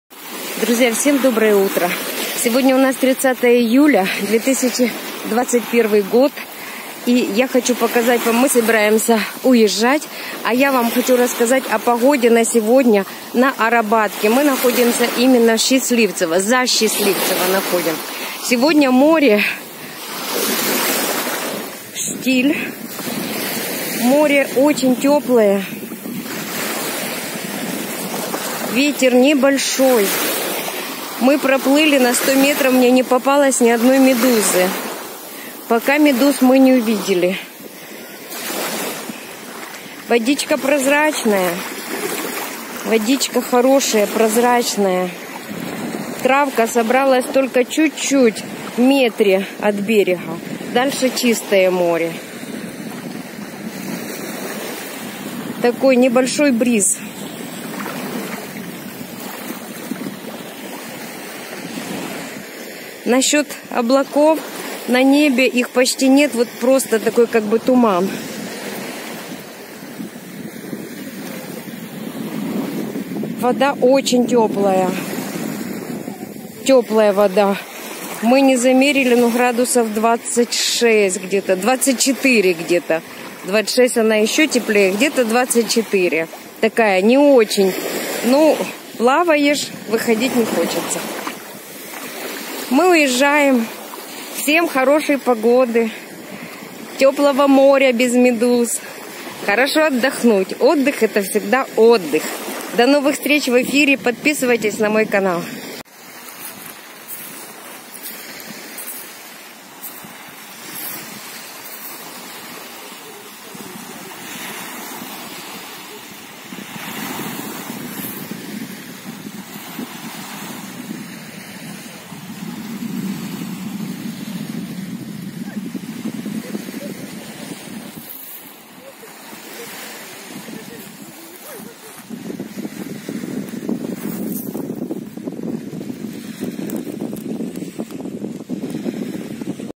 31 июля 2021 море Азовское стрелка Арабатка.погода. море.
погода на Азовском море и медуз пока нет